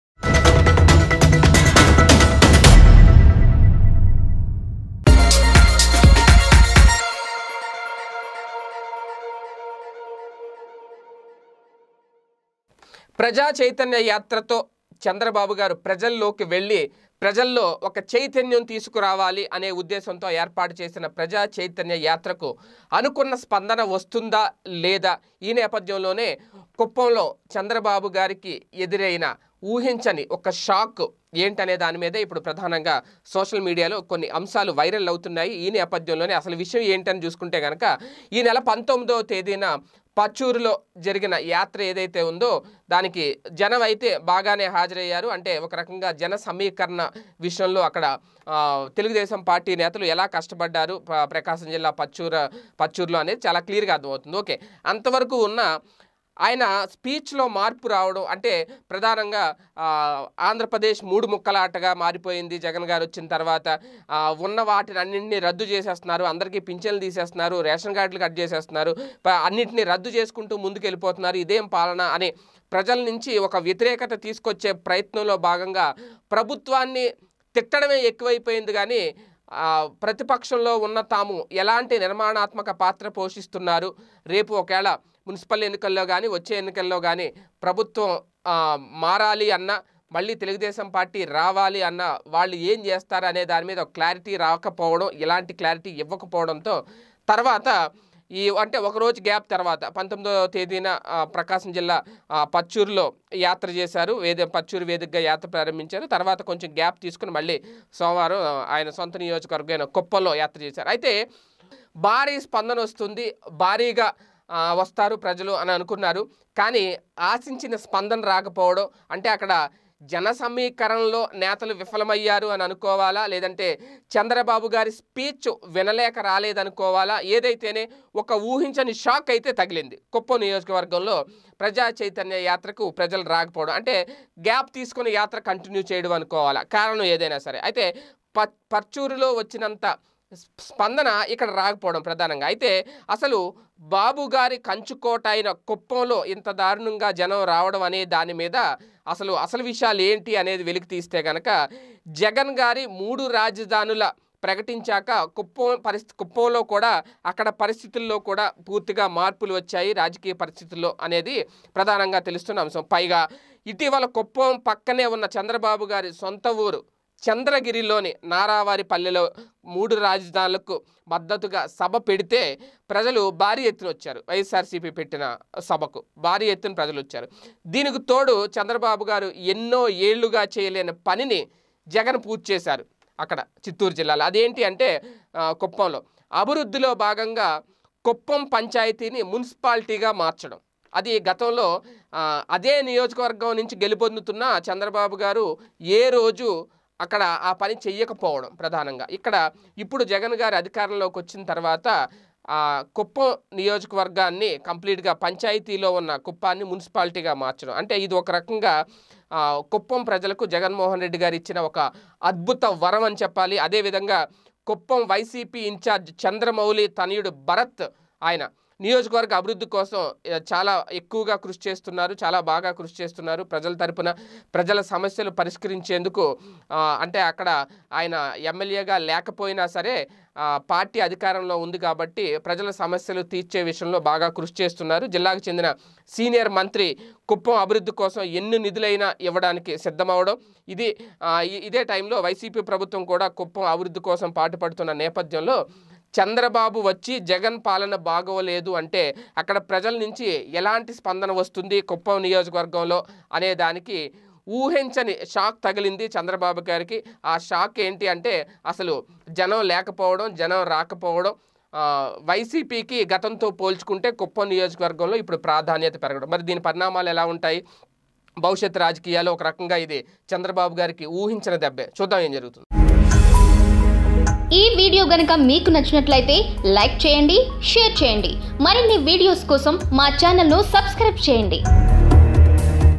Kuppam Public Shock to Chandrababu | కుప్పంలో చంద్రబాబుకు గట్టి దెబ్బ.. చాప్టర్ క్లోజ్..!
కుప్పంలో చంద్రబాబుకు గట్టి దెబ్బ.. చాప్టర్ క్లోజ్..!  | Kuppam Public Shock to Chandrababu